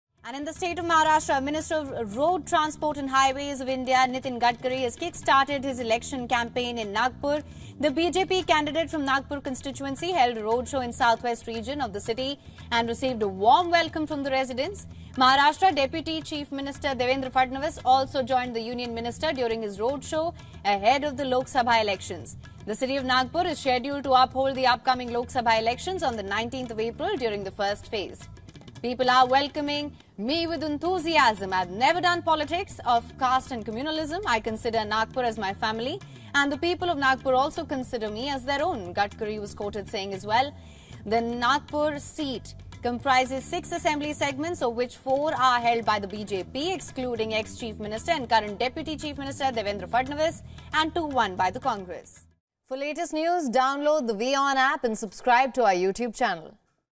India Elections: Nitin Gadkari holds mega rally in Nagpur, kicks off election campaigning | WION
Minister of Road, Transport & Highways - Nitin Gadkari - has started his election campaigning in Nagpur. The BJP candidate from Nagpur constituency held a road show in Southwest region.